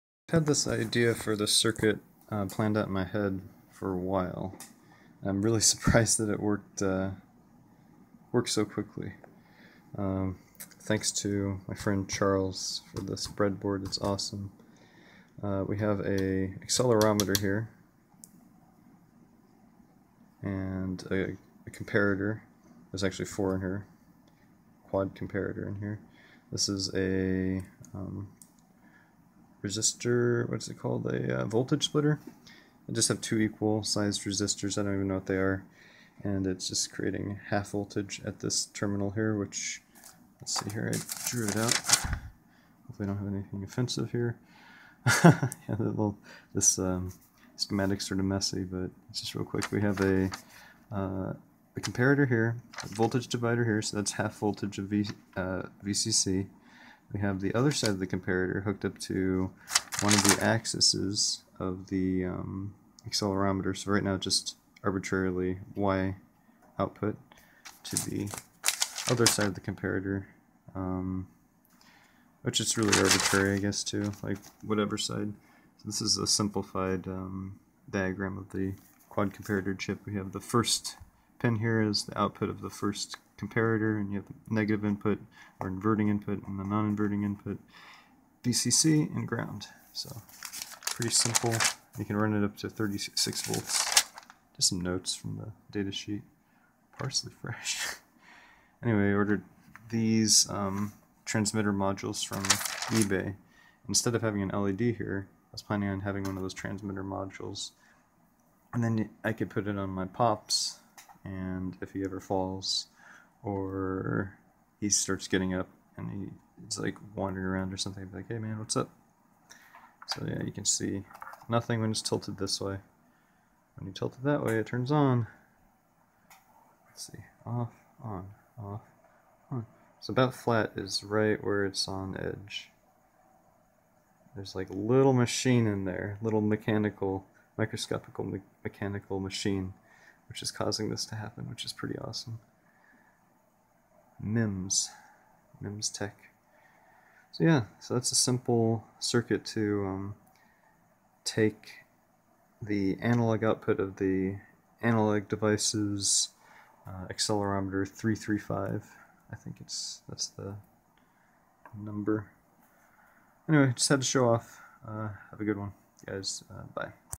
Accelerometer trigger circuit
ADXL335 connected to LM239N comparator out to an LED

Mainly going for a simple fall detection, activity alerting device.  I want to stay as analog/discrete as I can.

I also have ideas for building an analog synth that uses voltage controlled whatevers that get modulated with the accelerometer.